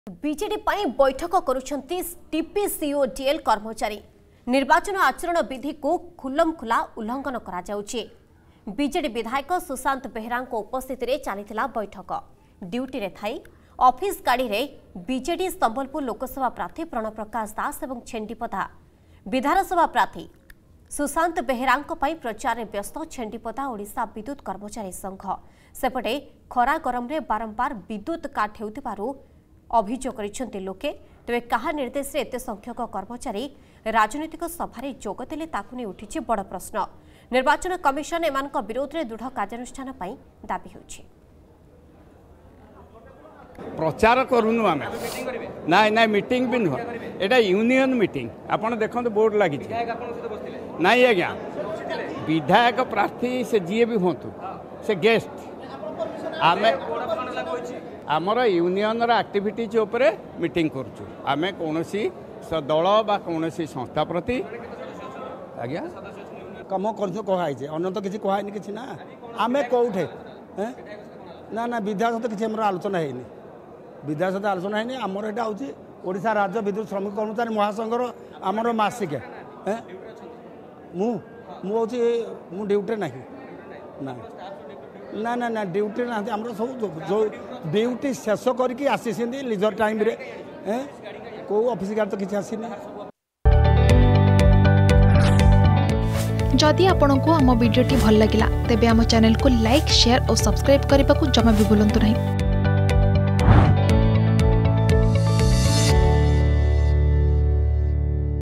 ବିଜେଡି ପାଇଁ ବୈଠକ କରୁଛନ୍ତି TPCODL କର୍ମଚାରୀ || BJD Meeting TPCODL
ବିଜେଡି ପାଇଁ ବୈଠକ କରୁଛନ୍ତି TPCODL କର୍ମଚାରୀ । ନିର୍ବାଚନ ଆଚରଣ ବିଧିକୁ ଖୋଲମ ଖୋଲା ଉଲ୍ଲଙ୍ଘନ।  ।